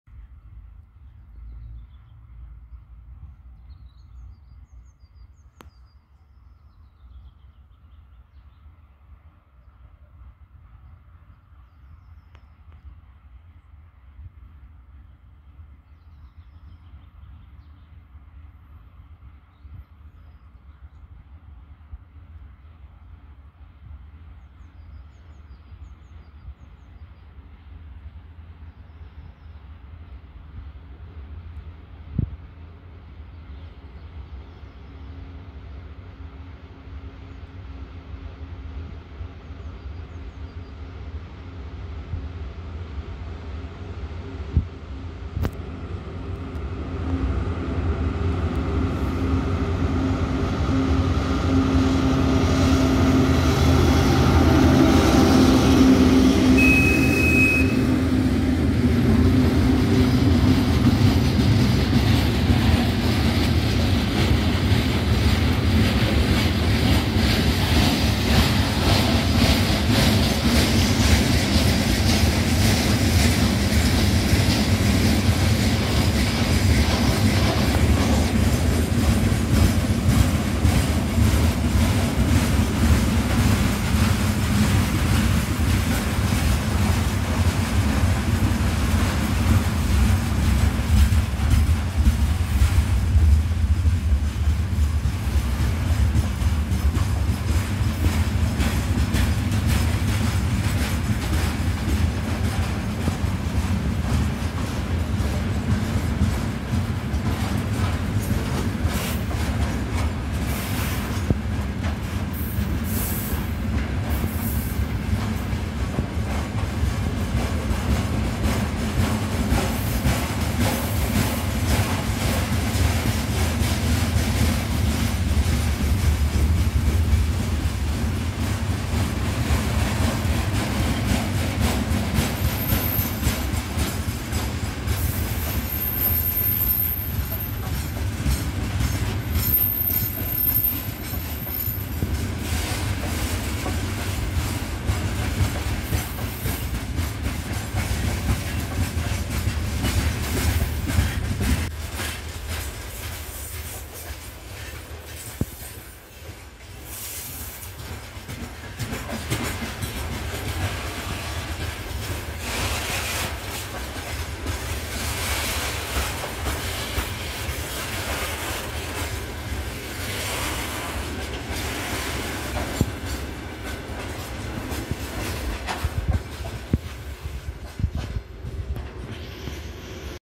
2М62УМ-0266|2M62UM-0266
[RU] 2М62УМ-0266 Едет в сторону станции Саркандаугава

[LV] 2M62UM-0266 brauc Sarkandaugavas stacijas virzienā

[EN] 2M62UM-0266 heading towards Sarkandaugawa station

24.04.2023